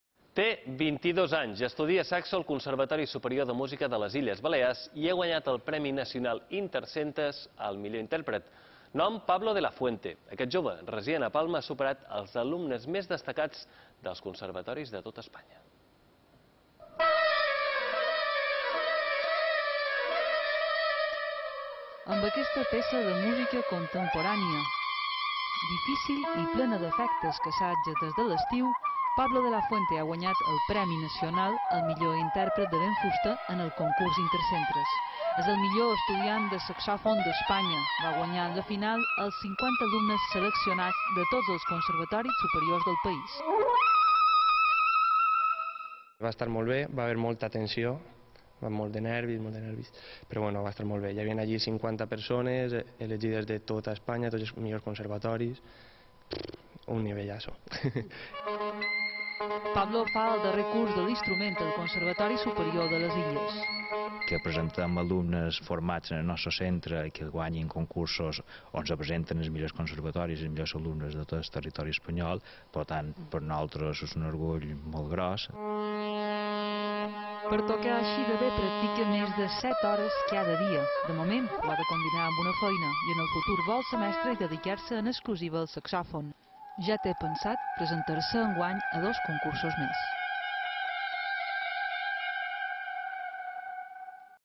Pablo de la Fuente, el millor estudiant de saxòfon d’Espanya
Té 22 anys, estudia Saxo al Conservatori Superior de Música de les Illes Balears i ha guanyat el premi nacional intercentres al millor intèrpret.